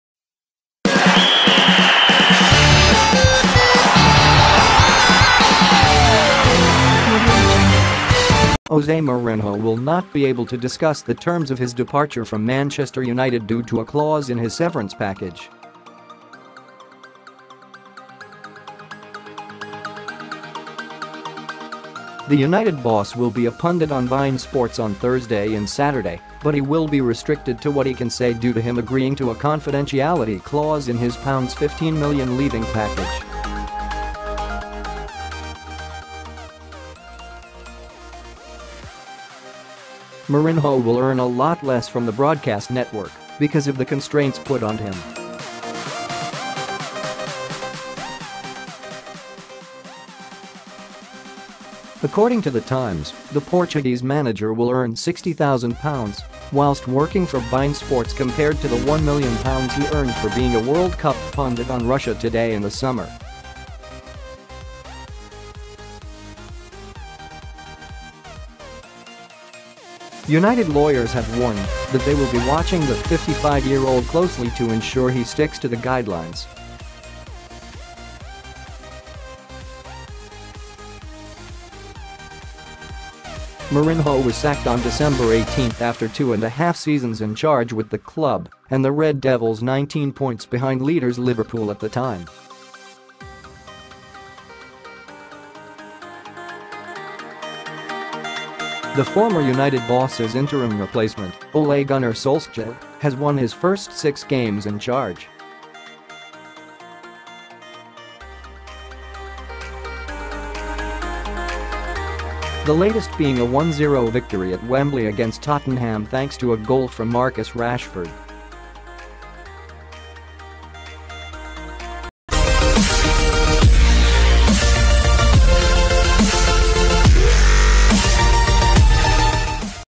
Jose Mourinho gagged by United ahead of appearance on beIN SPORTS
Jose Mourinho will not be able to discuss the terms of his departure from Manchester United due to a clause in his severance package. The United boss will be a pundit on beIN SPORTS on Thursday and Saturday but he will be restricted to what he can say due to him agreeing to a confidentiality clause in his £15million leaving package. Mourinho will earn a lot less from the broadcast network because of the constraints put on him.   According to the Times, the Portuguese manager will earn £60,000 whilst working for beIN SPORTS compared to the £1m he earned for being a World Cup pundit on Russia Today in the summer. United lawyers have warned that they will be watching the 55-year-old closely to ensure he sticks to the guidelines.  Mourinho was sacked on December 18 after two and a half seasons in charge with the club and the Red Devils 19 points behind leaders Liverpool at the time. The former United boss' interim replacement, Ole Gunnar Solskjaer, has won his first six games in charge.The latest being a 1-0 victory at Wembley against Tottenham thanks to a goal from Marcus Rashford.